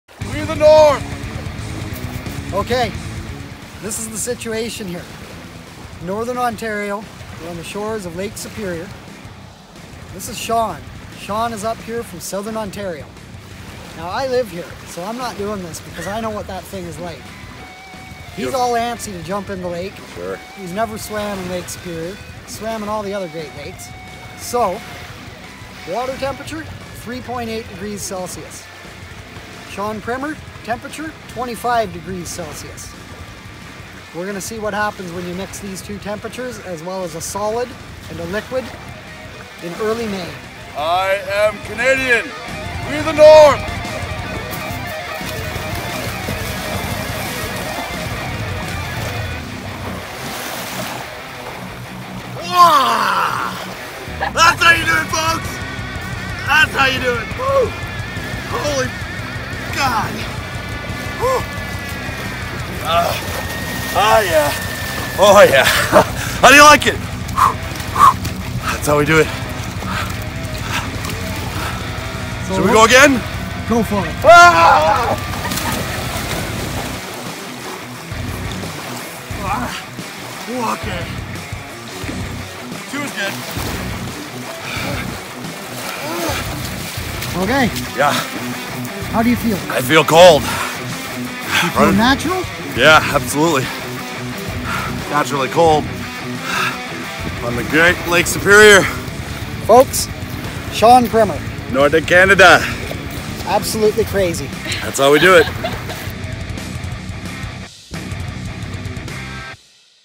Crazy Canuck Jumps Into FRIGID Lake - Lake Superior Provincial Park - May 2019
Our friend Shawn is visiting from southern Ontario.  He's been itching to jump into lake Superior since he got here.  It's early May, 2019 and the lake temperature is currently 3.8*C.  Thats pretty chilly!  Lets see what happens when we take him to a beautiful beach on the lake superior coastal trail in Lake Superior Provincial Park.

Our Amazon Wish List